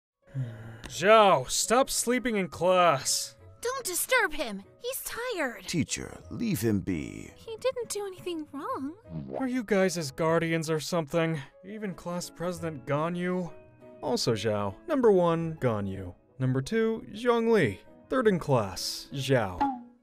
Xiao Has A Sleep Protection Squad (genshinimpact Comic Dub)
Without the comic artist this video wouldn't exist, so I'd appreciate if you could check them out

Narrator, Teacher, and Xiao - @Kyotosomo
Venti and Ganyu - @HeatherSommers
Zhongli - @MarsVA
Editor - @Aishlie